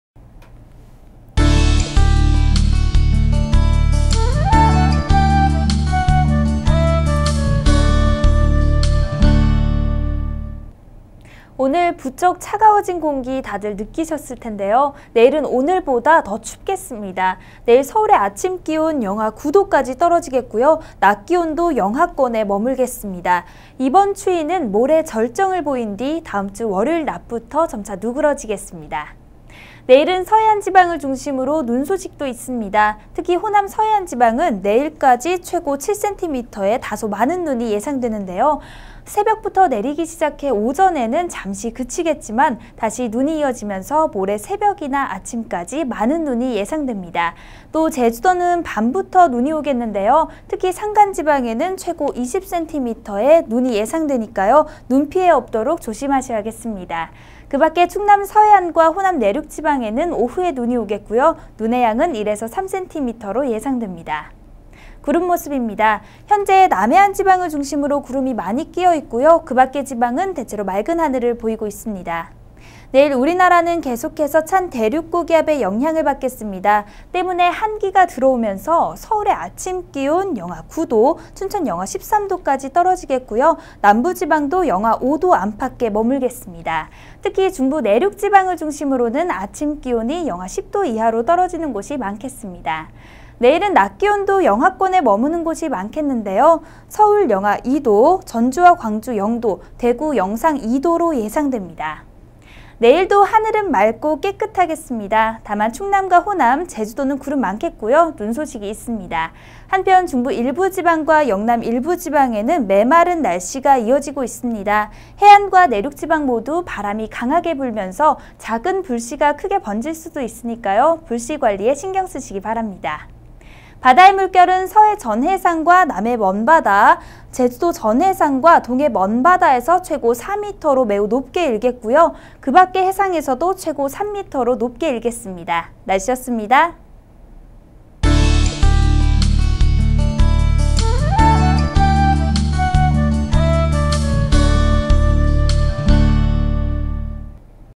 [날씨정보 2012년 2월 16일 23시]내일(금) 전국 대체로 맑음
▶내일(금) 전국 맑음(충남*호남*제주도 구름 많음)
... 기온↓, 찬바람
... 눈소식, 새벽: 호남서해안/ 오후: 호남내륙*충남서해안/ 밤: 제주도
... 경기동부내륙*강원내륙*충북내륙*경북북부내륙 지방 한파특보
   (중부내륙 중심, 아침기온 -10도↓)
... 중부*영남일부 건조주의보